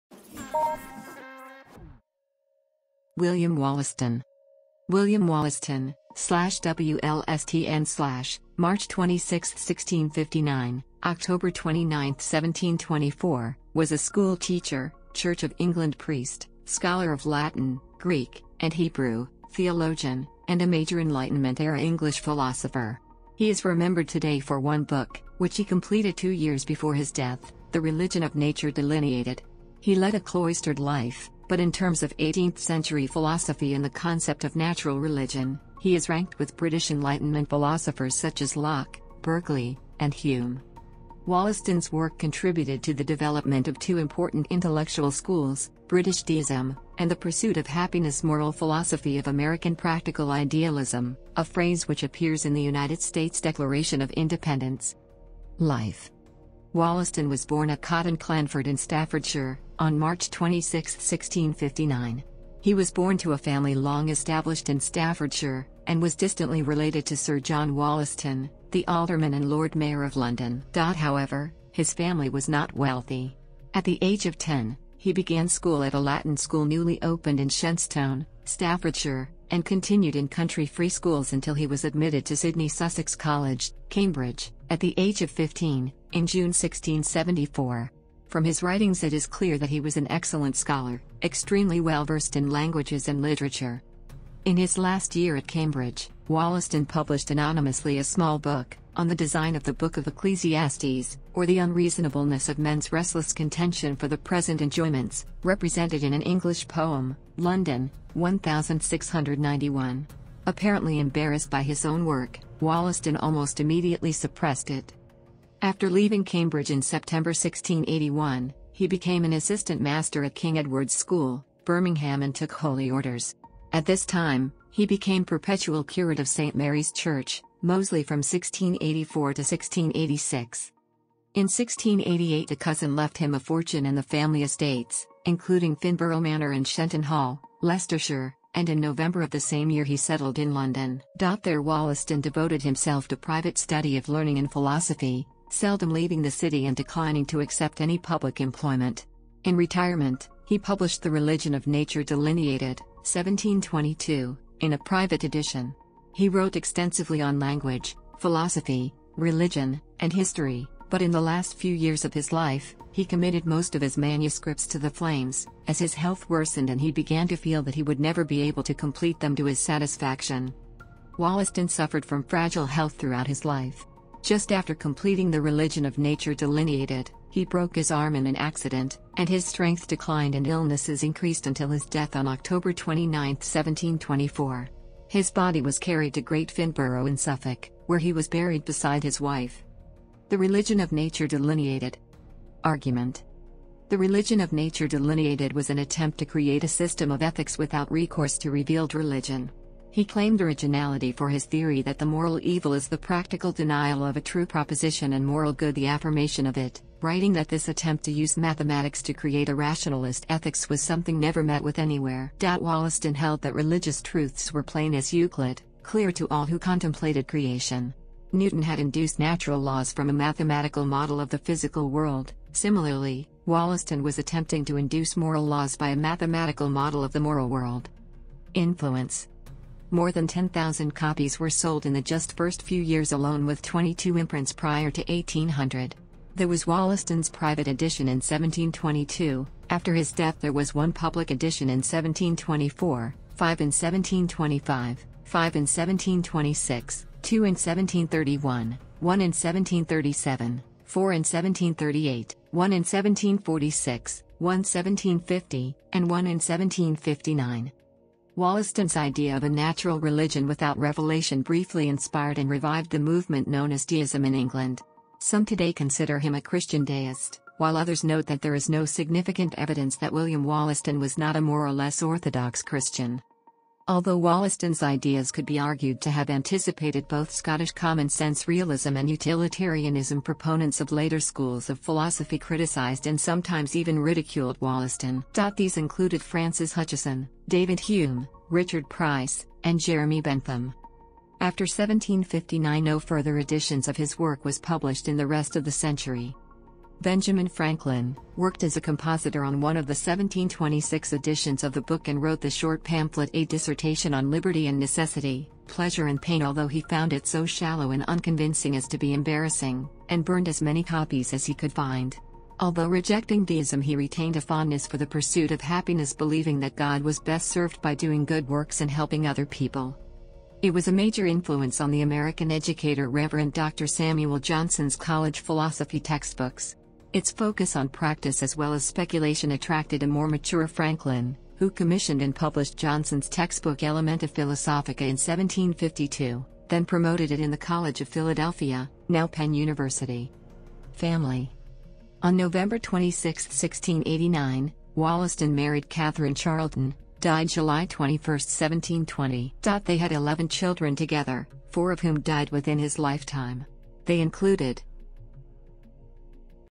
William Wollaston 👩‍🏫📜 Everything Philosophers 🧠👨🏿‍🏫
Books on Philosphy
Philosophy Books for Kids
Explore the world with this Digital Microscope
Explore the sky with this Telescope

William Wollaston (/ˈwʊləstən/; 26 March 1659 – 29 October 1724) was a school teacher, Church of England priest, scholar of Latin, Greek, and Hebrew, theologian, and a major Enlightenment era English philosopher. He is remembered today for one book, which he completed two years before his death: The Religion of Nature Delineated. He led a cloistered life, but in terms of eighteenth-century philosophy and the concept of natural religion, he is ranked with British Enlightenment philosophers such as Locke, Berkeley, and Hume.